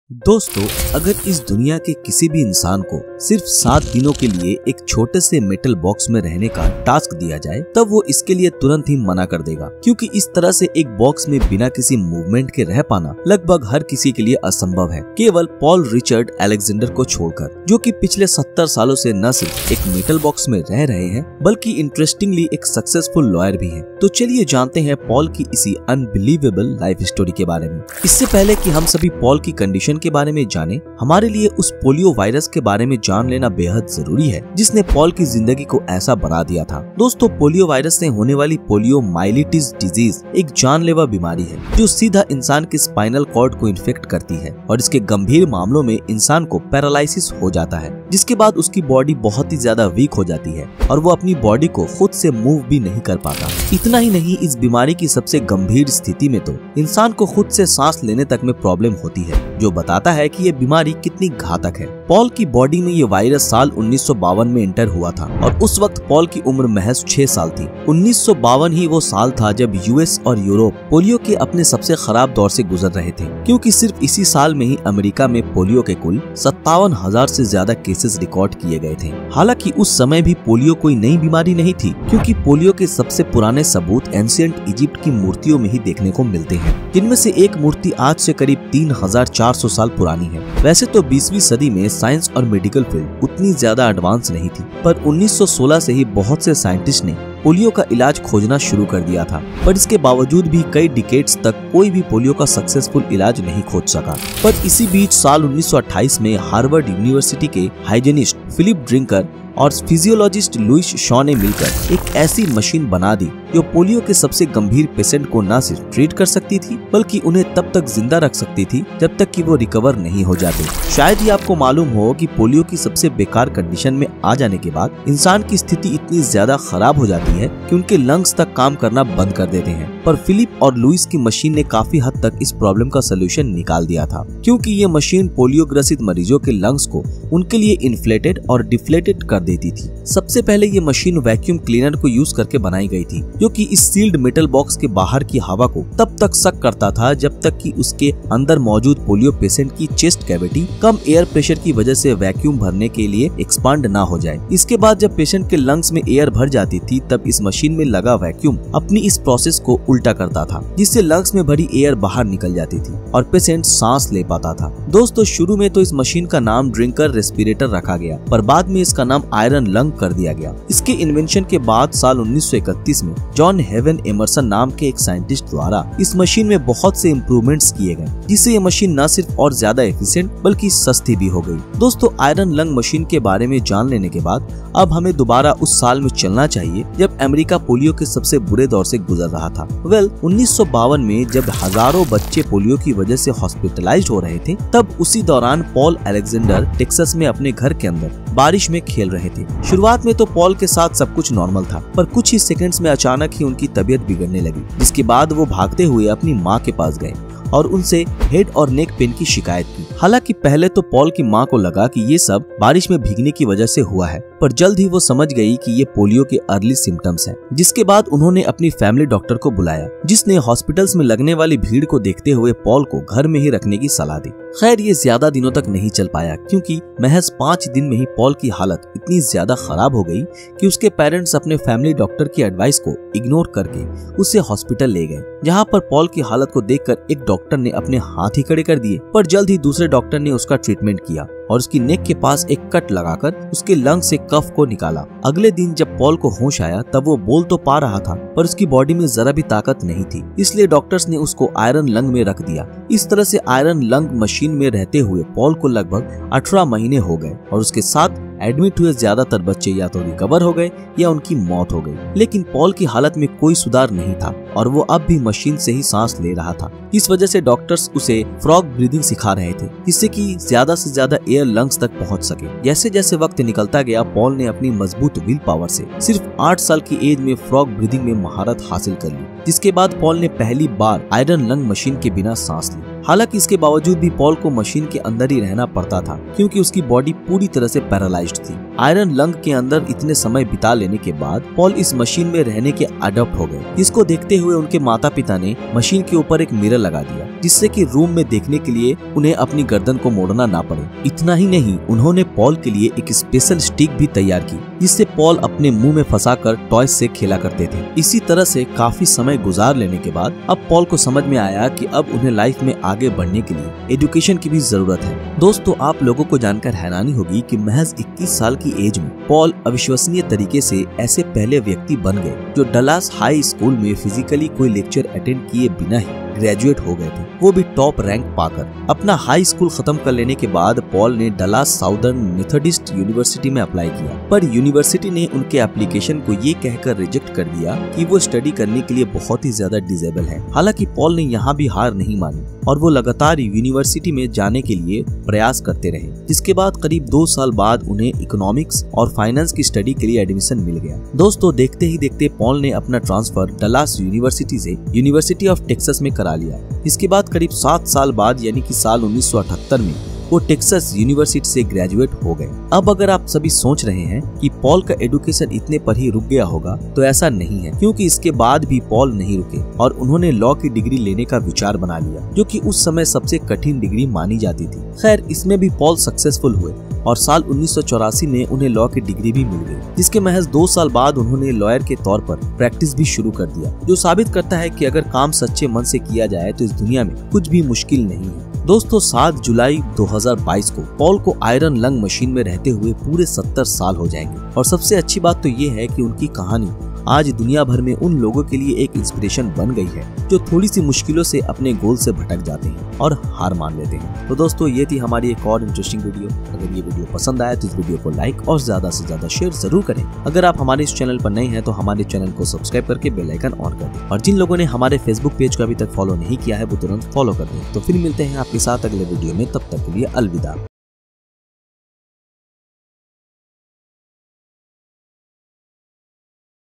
पिछले 70 सालो से ये इंसान जिस दर्द को झेल रहा है उसे सुनकर रोना आ जाएगा Paul Alexander's Sad Story
Paul Richard “Polio Paul” Alexander is a lawyer, writer and paralytic polio survivor. He is popularly known as one of the last people living in an iron lung after he contracted polio in 1952 at the age of six